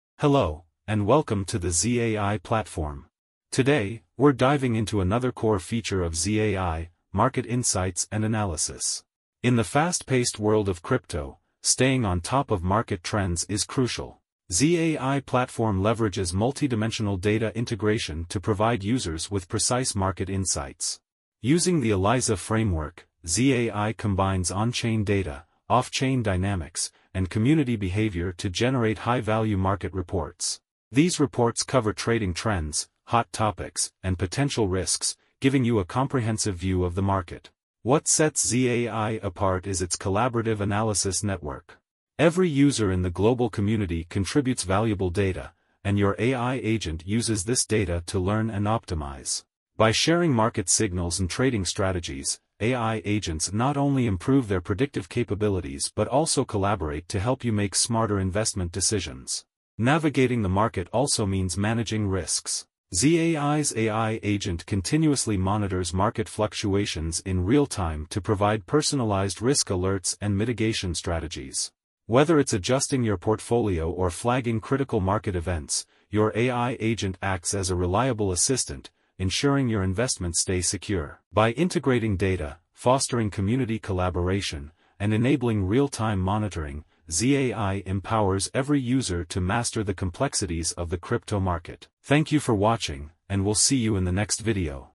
How ZAI AI Agents Revolutionize Crypto Market Analysis and Insights
ZAI platform redefines crypto trading with advanced market insights and real-time risk analysis. Powered by the ELIZA framework, ZAI integrates on-chain data, off-chain dynamics, and community behavior to deliver precise market reports.
Personalized AI Agents continuously learn and collaborate within a global network, optimizing strategies and predicting trends. From trading signals to risk alerts, ZAI empowers users with actionable insights to navigate the complexities of the crypto market.
Stay ahead with ZAI’s innovative tools and make smarter investment decisions with confidence.